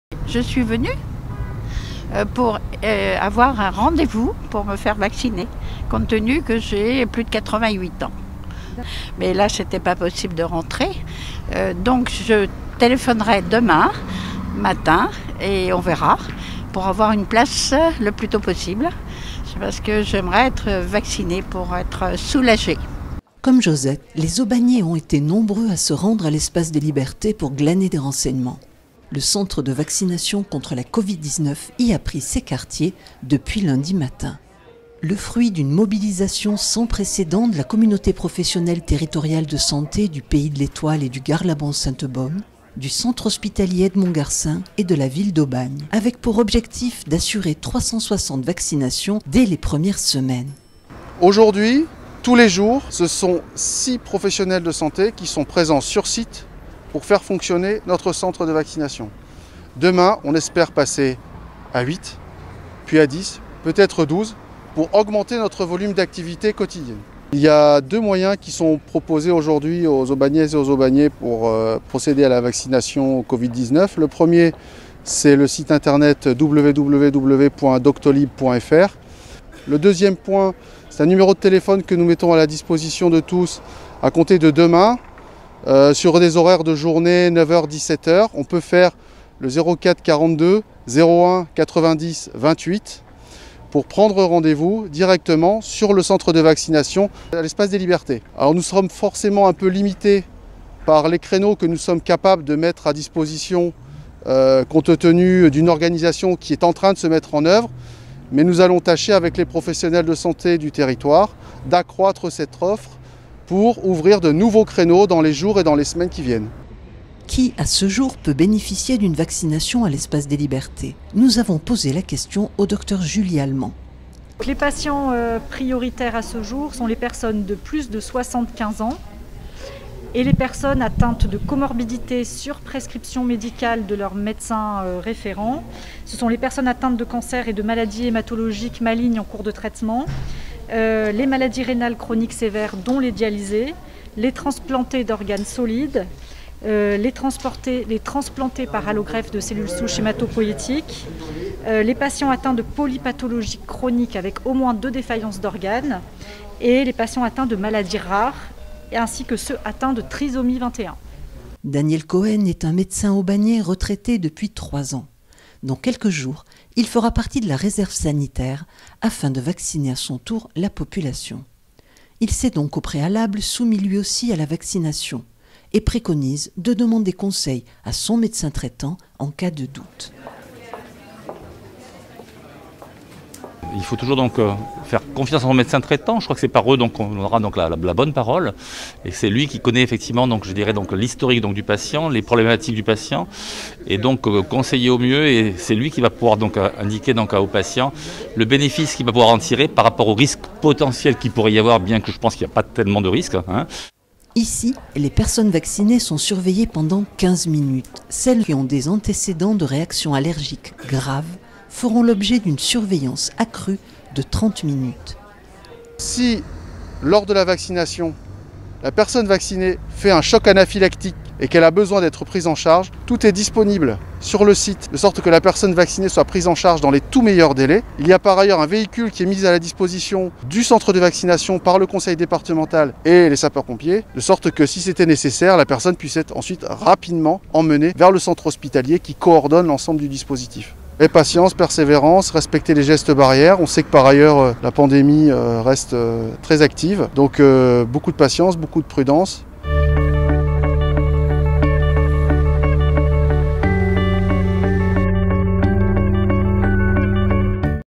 Ouverture du centre de vaccination contre la Covid 19 à Aubagne
Depuis lundi 18 janvier,  la Ville d'Aubagne accueille un centre de vaccination contre la Covid-19 au sein de l'Espace des Libertés. Le fruit de la mobilisation du centre hospitalier d'Aubagne, accompagné par les professionnels de santé libéraux du Pays d'Aubagne et de l'Etoile ainsi que ceux du Garlaban.
Reportage sur la toute première journée de vaccination dédiée aux professionnels de santé.

A noter que pour toute prise de rendez-vous, il convient de composer le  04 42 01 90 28.